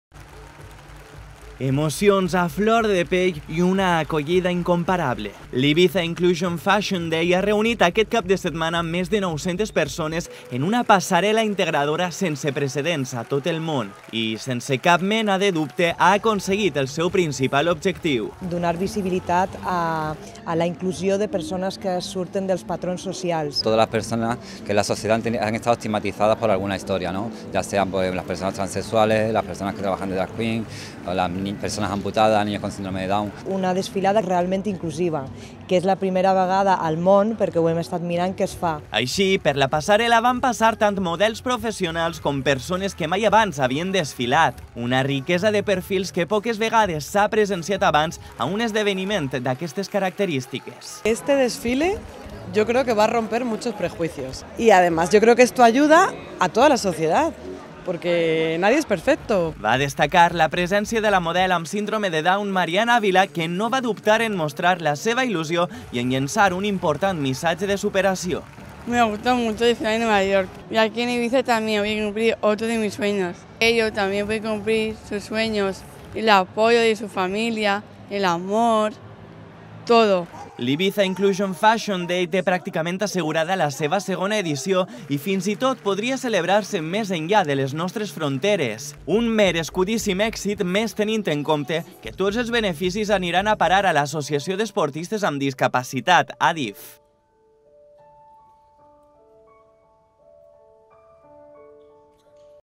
09/03/2020 La primera passarel·la inclusiva del món
Dissabte es va celebrar al Recinte Firal d’Eivissa la primera edició de l’ ‘Ibiza inclusion fashion day’. Més de 900 persones van assistir a aquesta desfilada de moda pionera al món pel seu caire integrador. L’esdeveniment destinarà la seva recaptació a ADDIF i ja té pràcticament assegurada una segona edició.